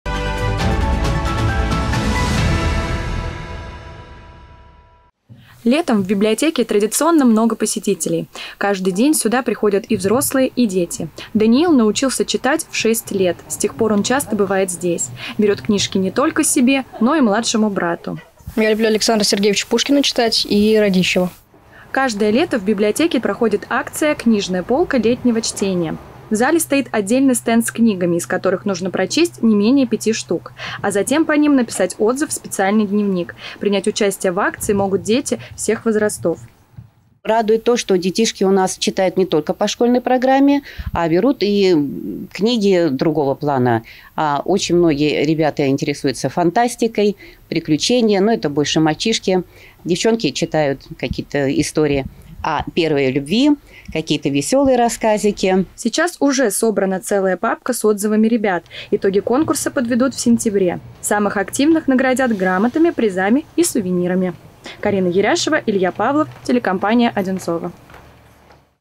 Книги на лето
Лето лучшее время для посещения библиотеки. Там можно не только прочитать любимую книгу, но и принять участие в различных мероприятиях. Подробнее о них в сюжете ОТВ.